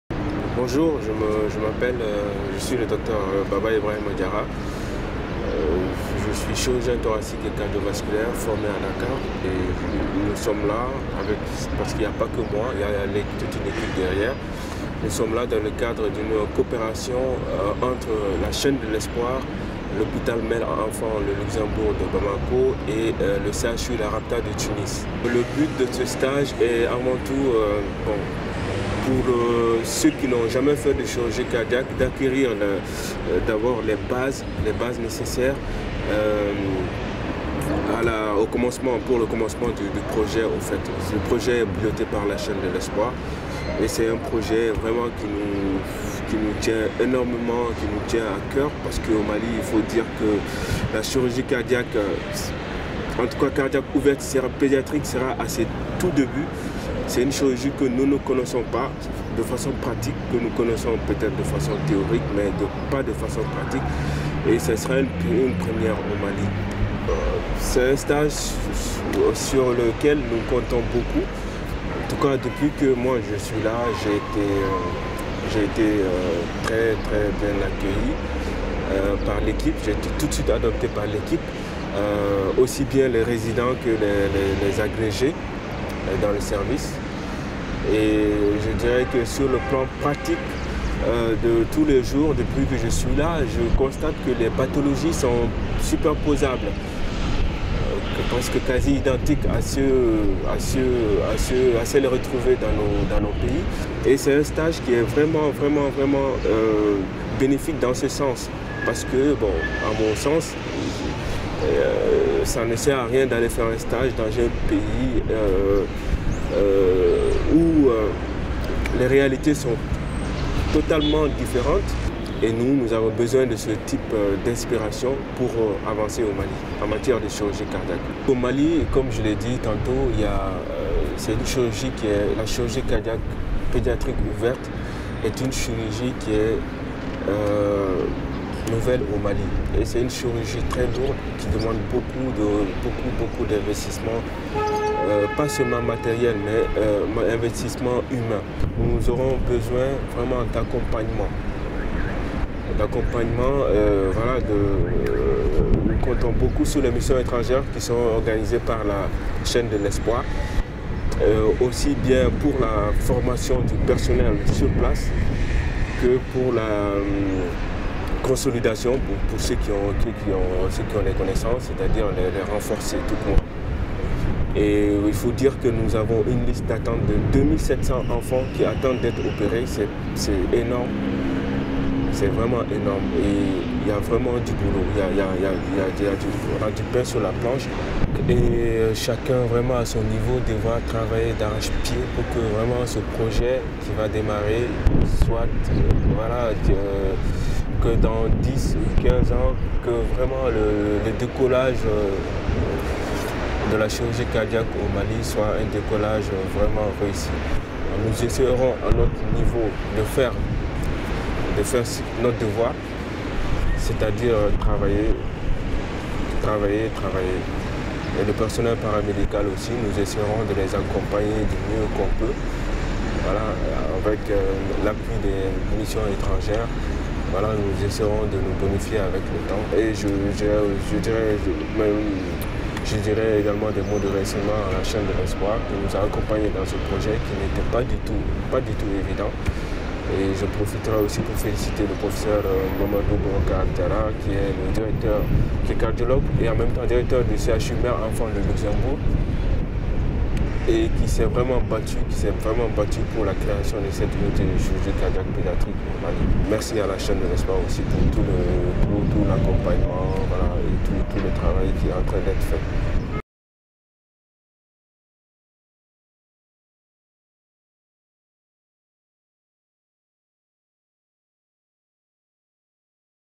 Interview du Dr Baba Ibrahima Diarra - Programme de formation au CHU La Rabta
En vue de la prise en charge des enfants cardiaques au Mali, dans le futur Centre Festoc dédié au sein de l’hôpital Mère Enfant "Le Luxembourg", une équipe malienne de 7 personnes a été identifiée pour effectuer un stage de formation de 3 à 6 mois au sein du CHU La Rabta à Tunis. 4 infirmiers et infirmières sont ainsi formés depuis mi-décembre et le seront jusqu’à mi-juin. 2 anesthésistes-réanimateurs ont effectué un stage de 3 mois aux côtés de leur homologues tunisiens, et 1 chirurgien est récemment arrivé pour effectuer 3 mois de formation dans ce service.

Découvrez l'interview du Dr Baba Ibrahima Diarra, chirurgien cardio-thoracique et vasculaire à l’Hôpital Mère Enfant Le Luxembourg à Bamako et stagiaire au CHU La Rabta.